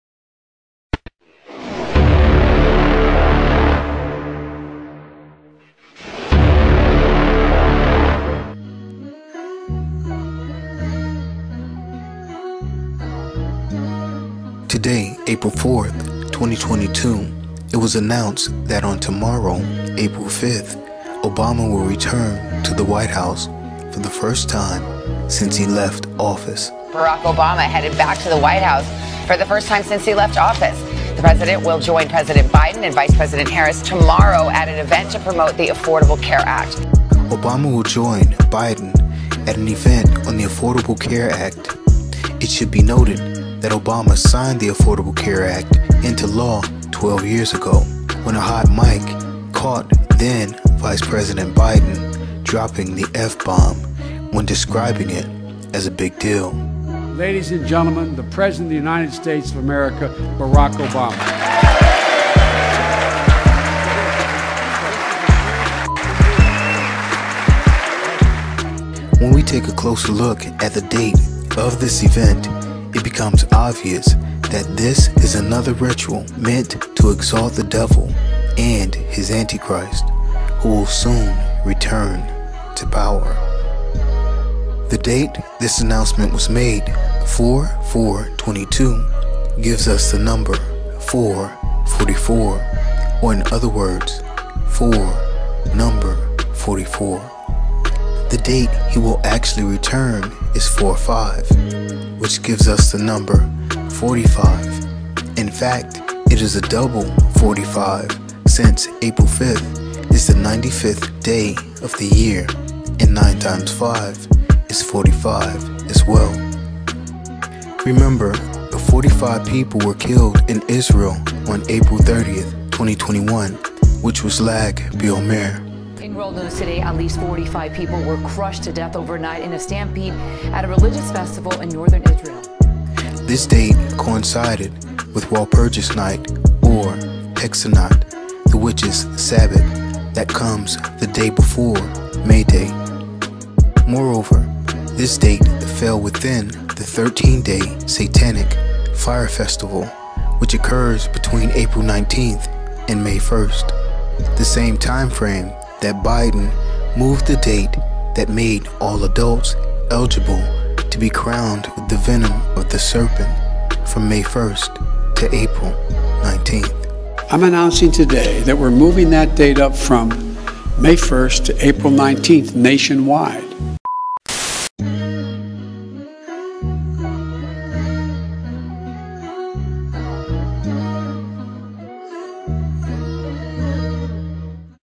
45 THE RETURN OF THE CHICAGO BULL LAUGHING LIKE THE DEVIL
Former President Barack Obama will join President Joe Biden at an event on the Affordable Care Act at the White House this week, a source familiar with the plans says, marking the first time the former President will appear publicly at the White House since leaving office. In this episode we will take a deeper look into this event in order to discover the hidden symbolism found within it.